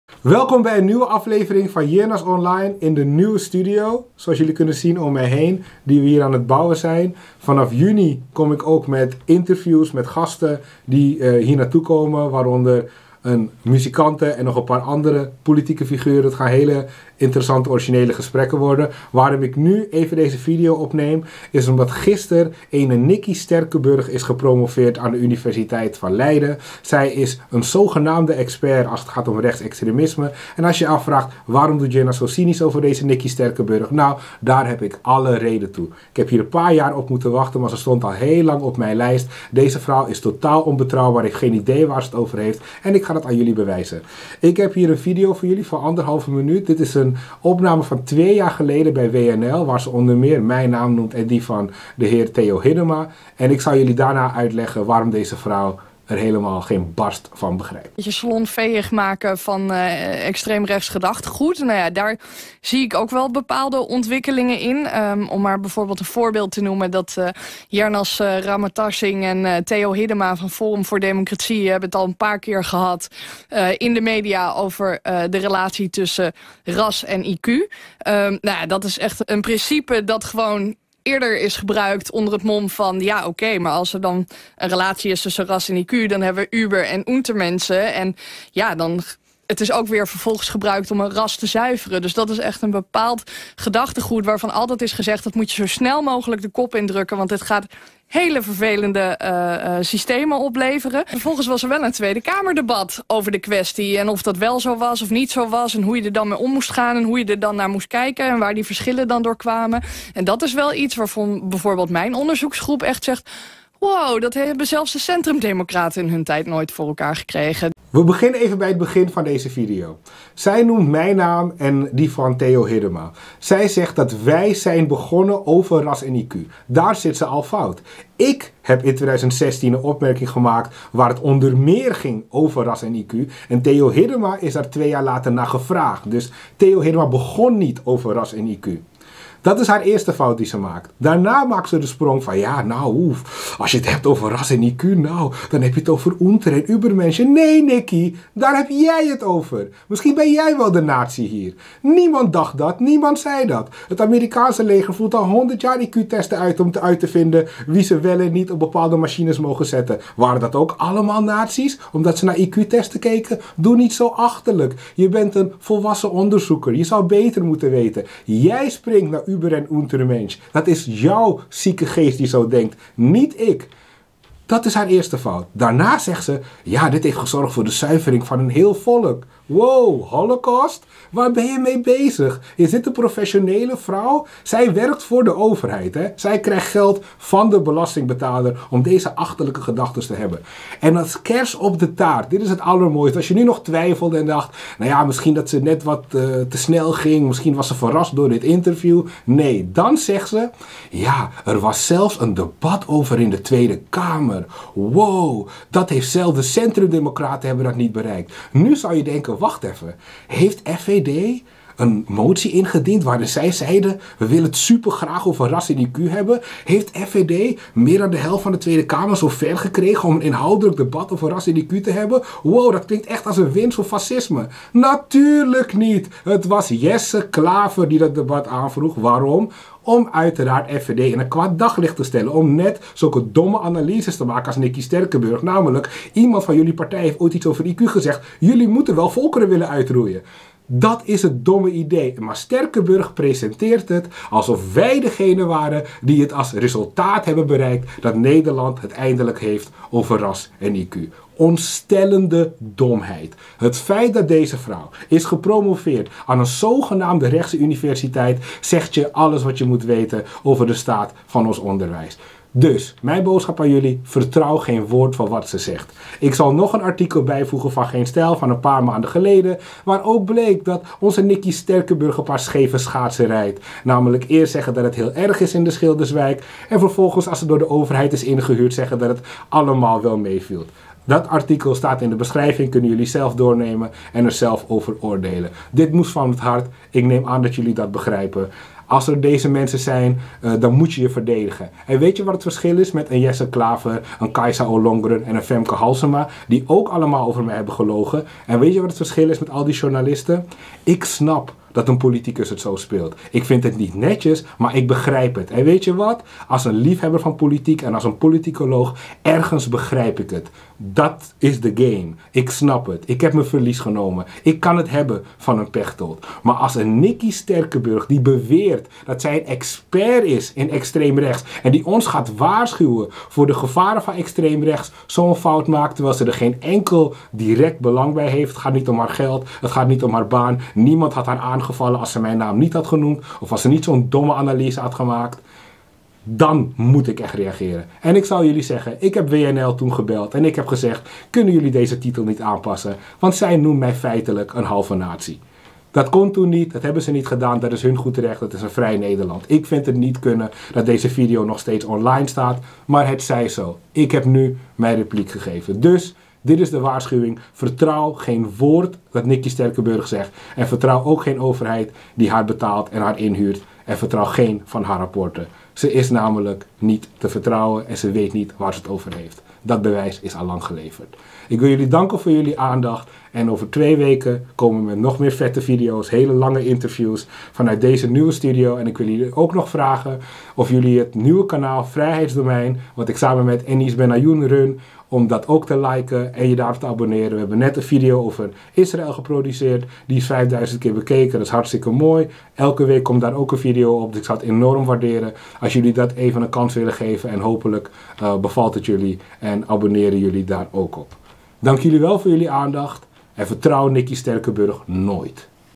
Yernaz reageert op promotie ''fake expert'' Nikki Sterkenburg
Mijn reactie op de promotie van Nikki Sterkenburg aan de Universiteit van Leiden, een ''zogenaamde'' expert op het gebied van extreem rechts. Waarom? dat leg ik uit in de video.
Dit artikel op Geenstijl uit November 2020 geeft ook geen fraai beeld van haar professionaliteit en objectiviteit. Kijk, lees en oordeel zelf.